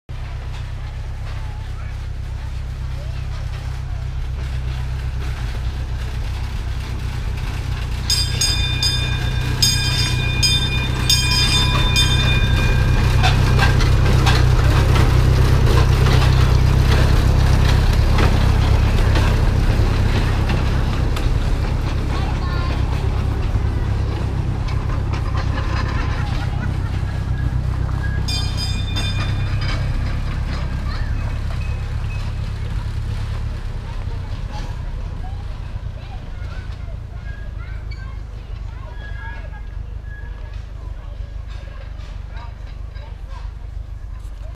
Sweetbrier, video 25, 4k
This is the train at the Sweetbrier Train and RV Park in Scio, Oregon.